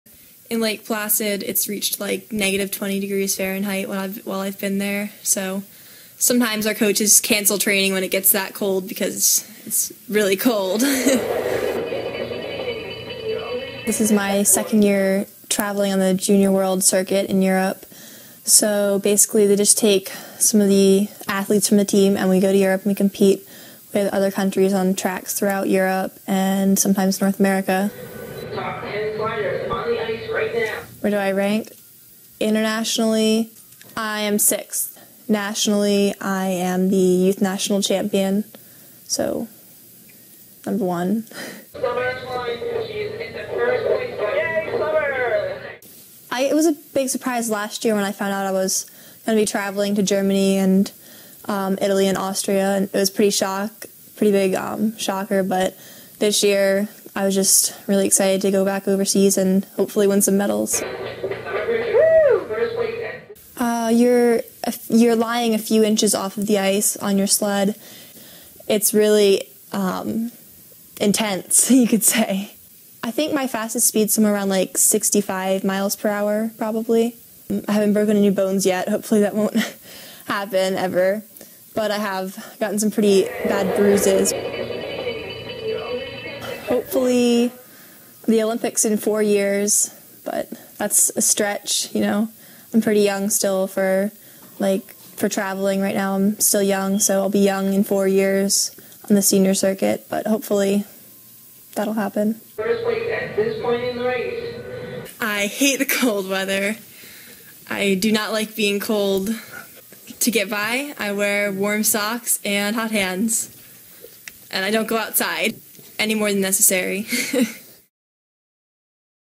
Summer shines in the winter
Summer Britcher of Glen Rock started luging two years ago and now finds herself the top-ranked youth in the country.